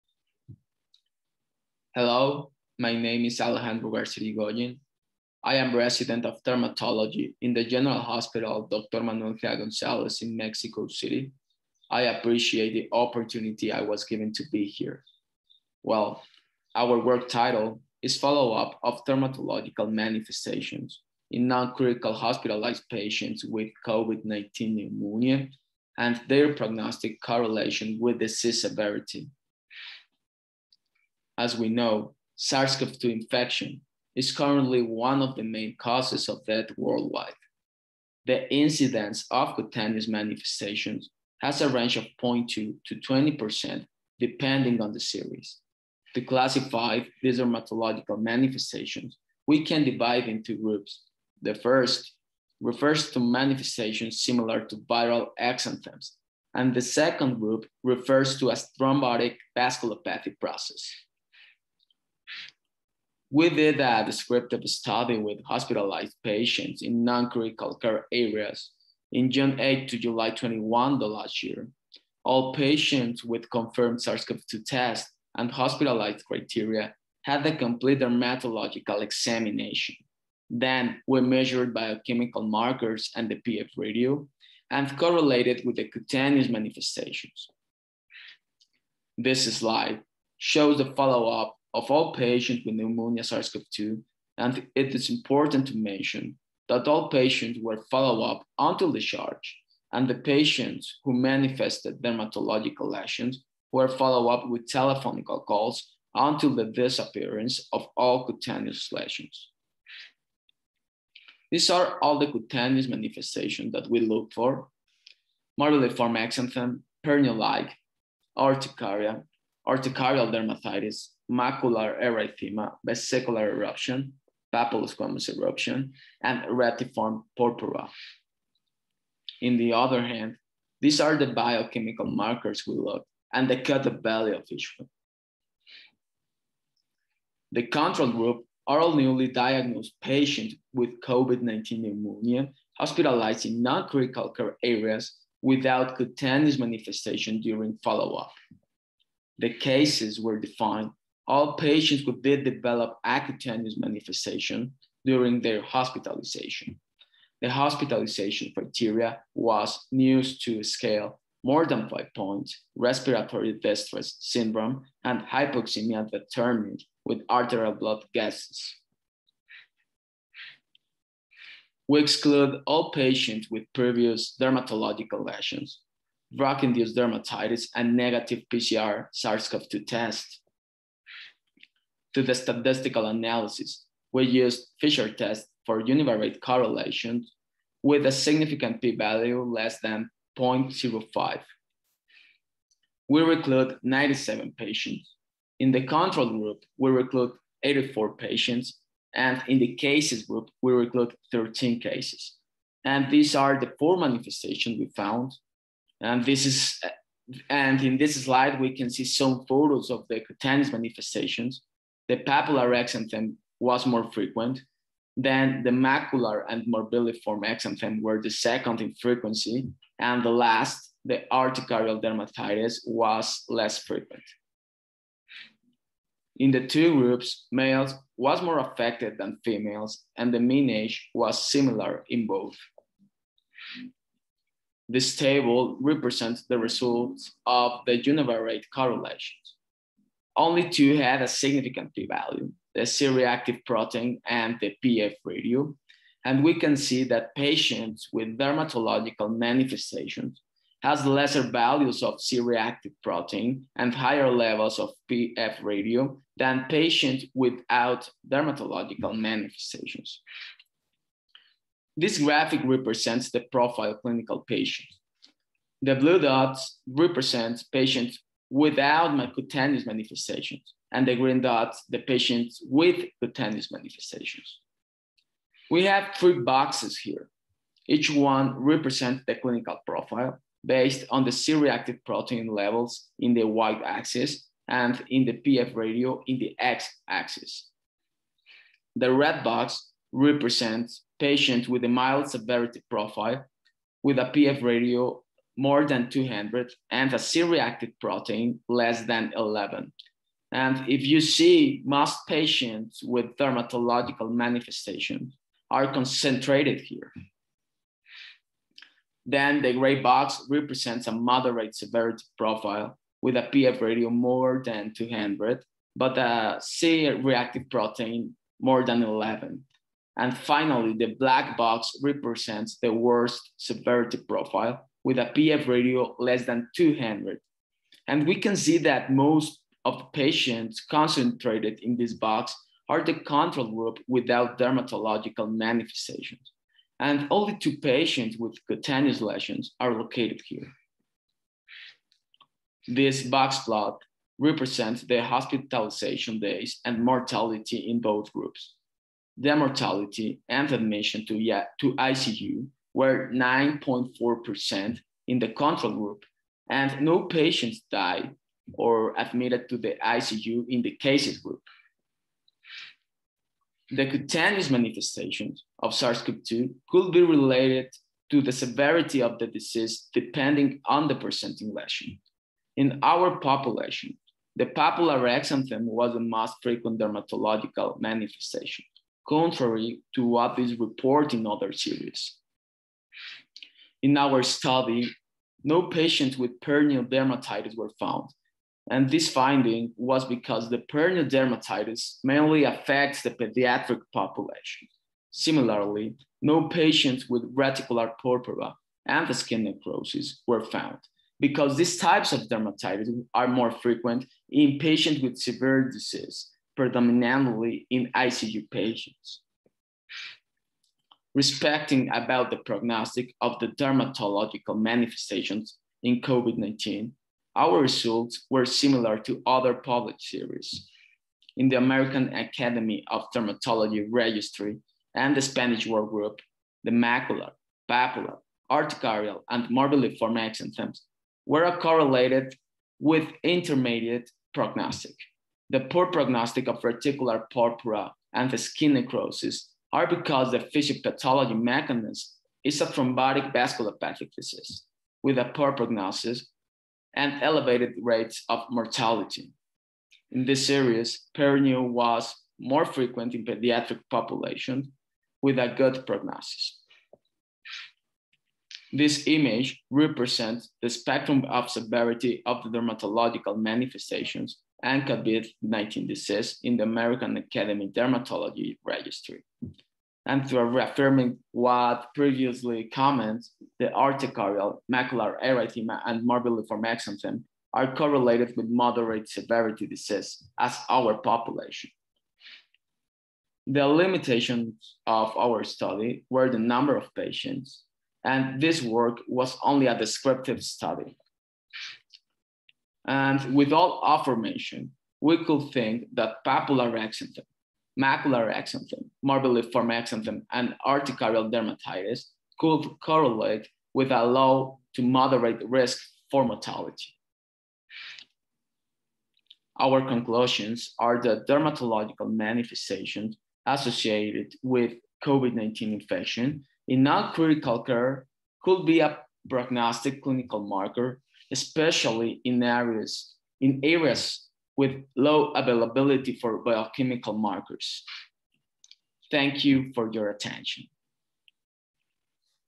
Alejandro García Irigoyen,Hospital General "Dr. Manuel Gea González,Mexico
Title: Follow-up of dermatological manifestations in non-critical hospitalized patients with COVID- 19 pneumonia and their prognostic correlation with disease severity.